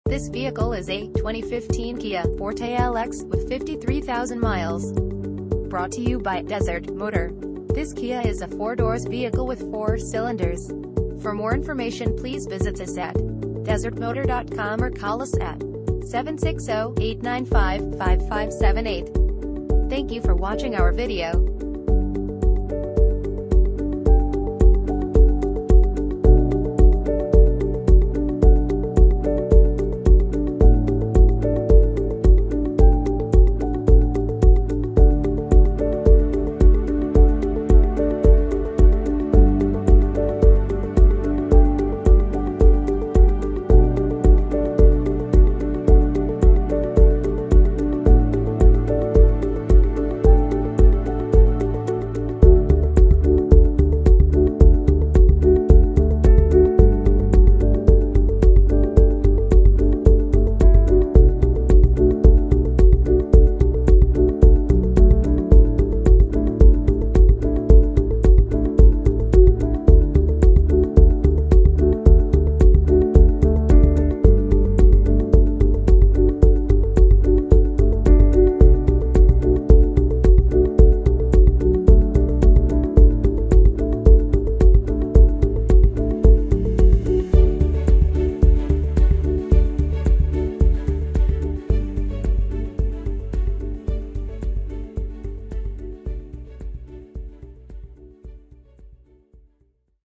2015 KIA Forte - DESERT MOTOR
DESERT MOTOR
41841 Beacon Hill

2015 KIA  Forte

One owner vehicle, Clean title, Manufacturer Warranty, 53,000 miles, Automatic, Sedan, FWD: Front Wheel Drive, 4 doors, 4 cylinders, Gasoline, MPG City 26 and 39 in Hwy.